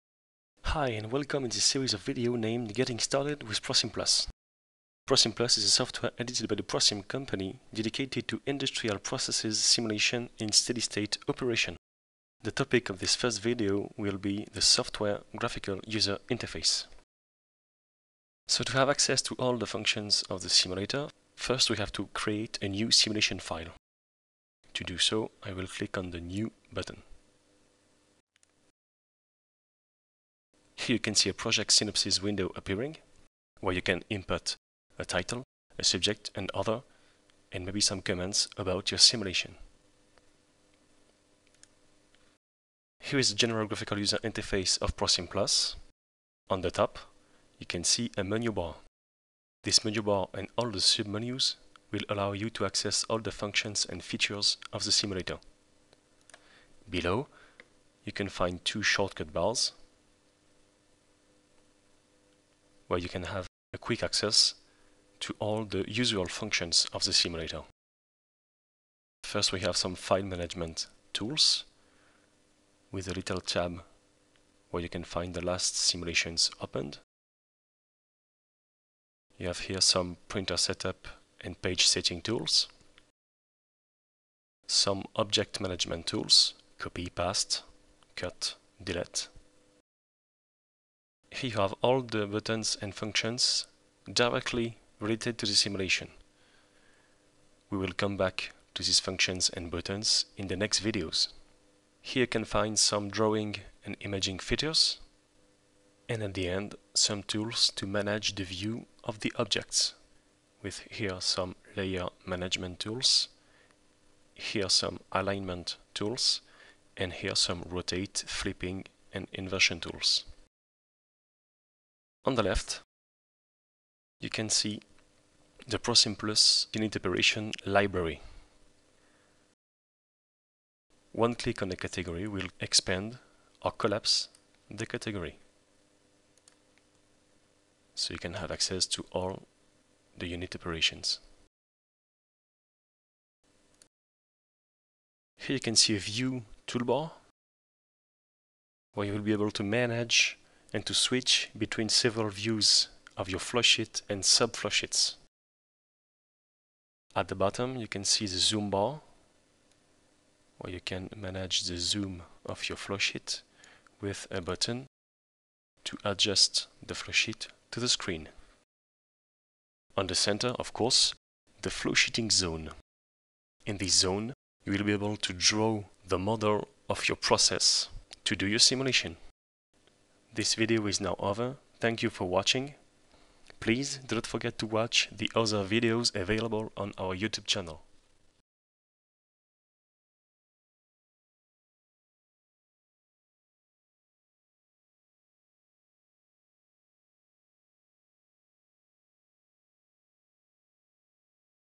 1. Getting Started with ProSimPlus: Graphical user interface - Process Simulation
Introduction to the graphical user interface of the ProSimPlus software, dedicated to the industrial process steady-state simulation.